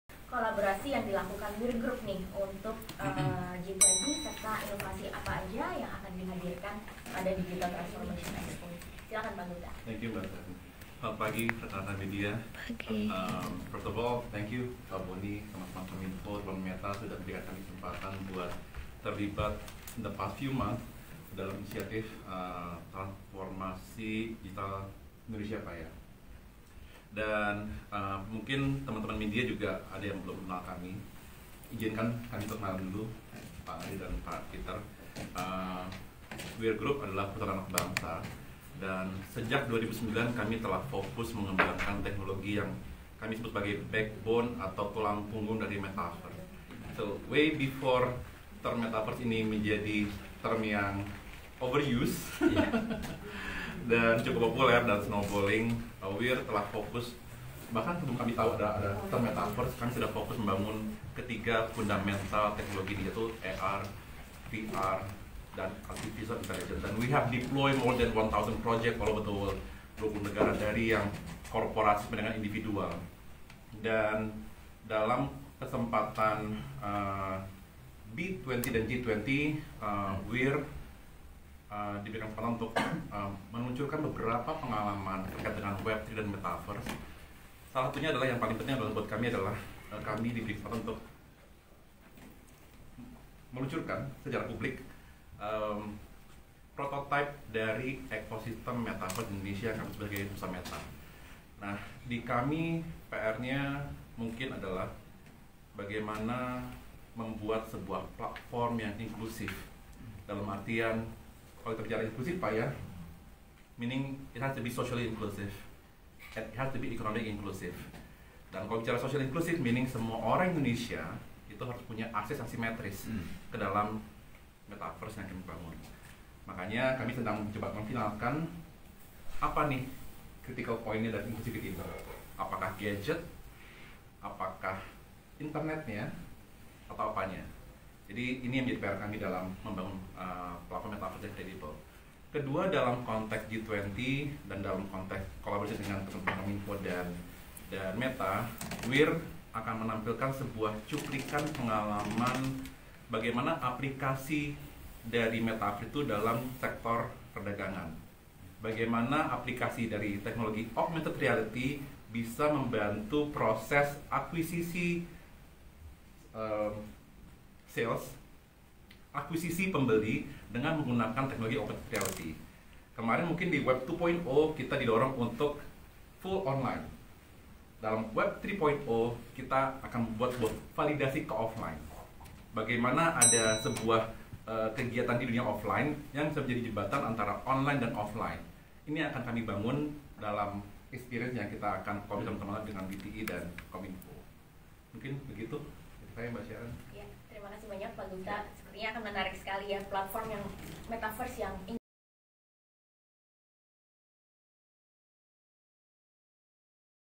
Bagaimana Kolaborasi WIR Group Untuk Presidensi G20 & Tranformasi Digital | NextLive
Nextren.com - Meta bersama Kementerian Komunikasi dan Informatika Republik Indonesia (Kominfo) serta WIR Group akan hadirkan Metaverse di KTT G20 di Bali.

Kehadiran eksebisi Metaverse pada KTT G20 bertujuan untuk menunjukan manfaat Metaverse ketika nanti hadir di dunia.

Eksebisi Metaverse yang dihadirkan oleh Meta, Kominfo, dan WIR Group, akan menunjukan sisi dua pilar.